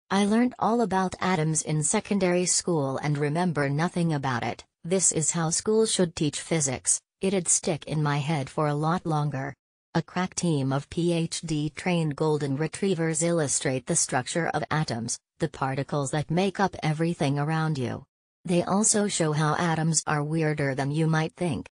16 Golden Retrievers Explain Atoms
I learnt all about atoms in secondary school and remember nothing about it, this is how schools should teach physics, it’d stick in my head for a lot longer!
“A crack team of PhD-trained golden retrievers illustrate the structure of atoms - the particles that make up everything around you. They also show how atoms are weirder than you might think.”